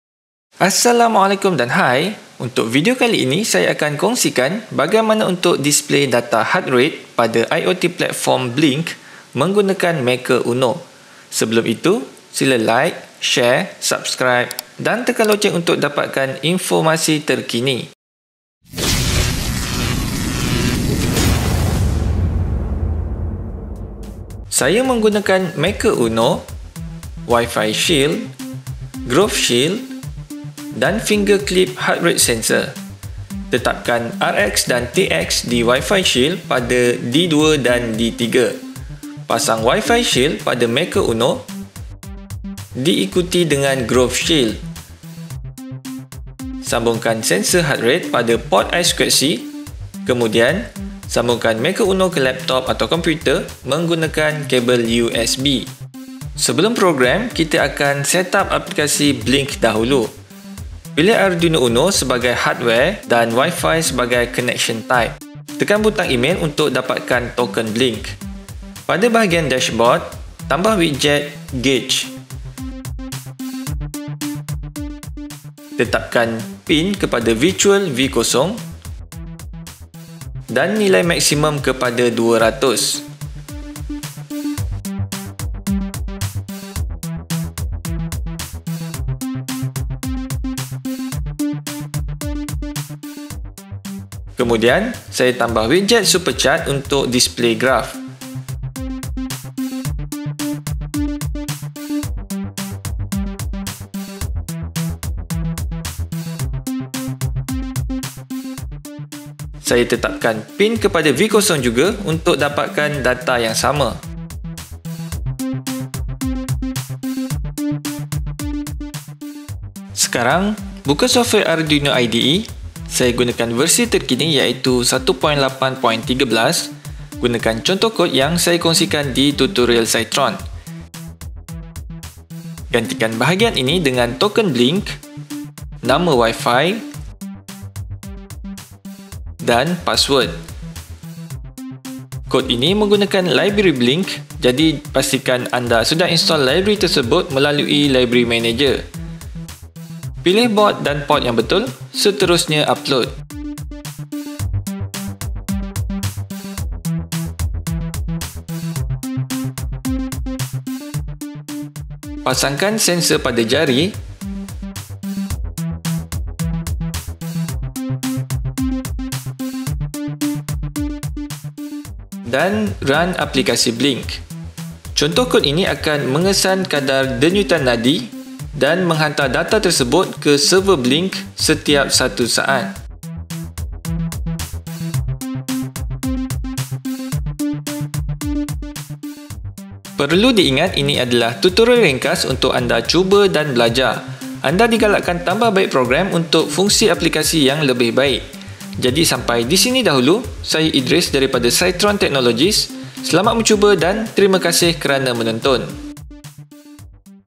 Monitoring Heart Rate Data on IoT Blynk App Using Arduino [BM]
This video shows how to display heart rate data to IoT platform Blynk using Maker UNO (Arduino Uno compatible).

and happy learning!